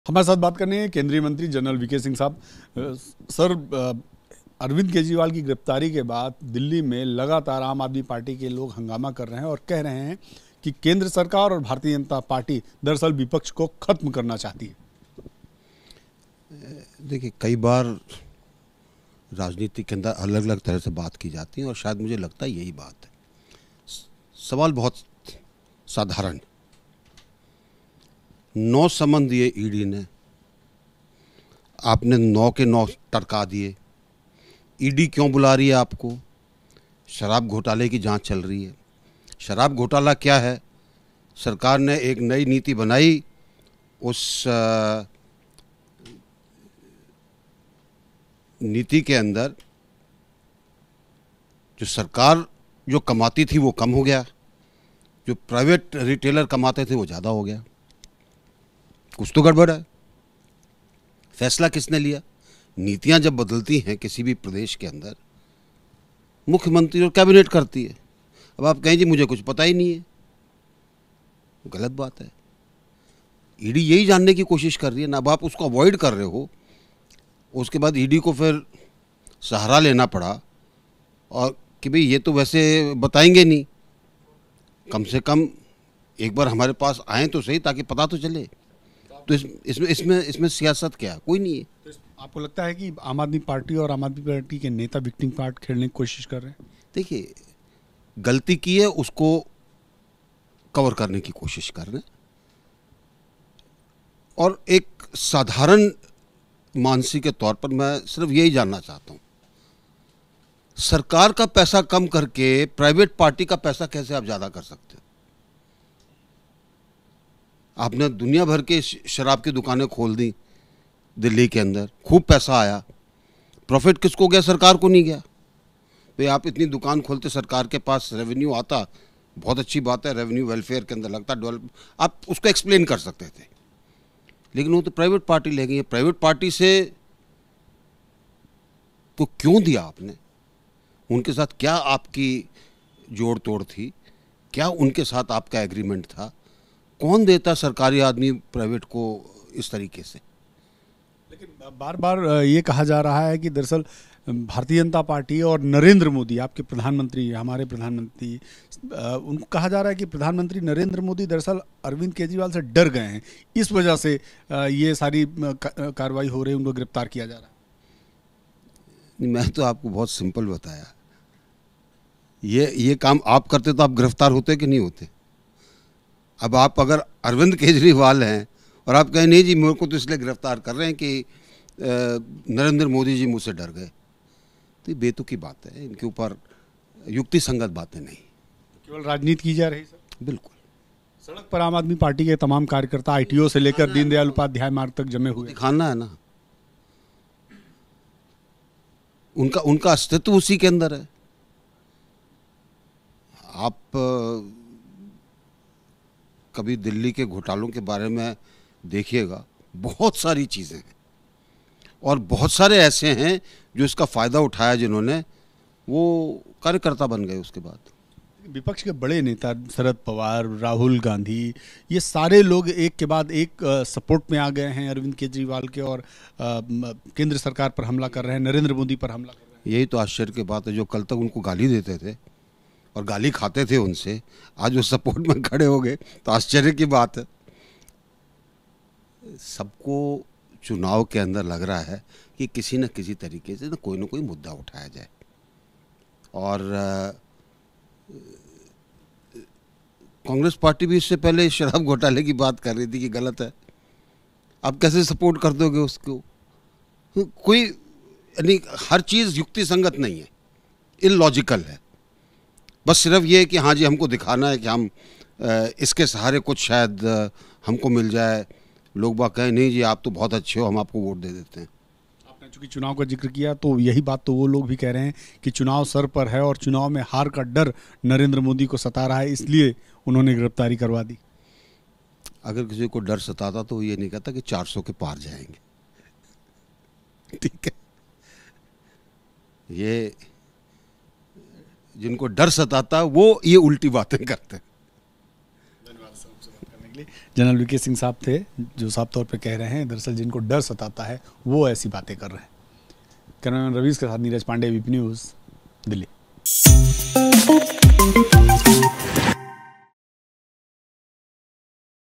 Arvind Kejriwal Arrested: 'AAP गलती करके छुपाने की कर रही कोशिश'- Vijay Kumar Singh | ABP News
Arvind Kejriwal Arrested:  दिल्ली के मुख्यमंत्री अरविंद केजरीवाल को प्रवर्तन निदेशालय (ईडी) ने गुरुवार रात गिरफ्तार कर लिया. सीएम की गिरफ्तारी का विरोध आप नेता ही नहीं कांग्रेस नेता भी कर रहे हैं. वहीं, बीजेपी नेता विजय कुमार सिंह ने आप पर तंज कसा. उन्होंने कहा- 'AAP गलती करके छुपाने की कर रही कोशिश'.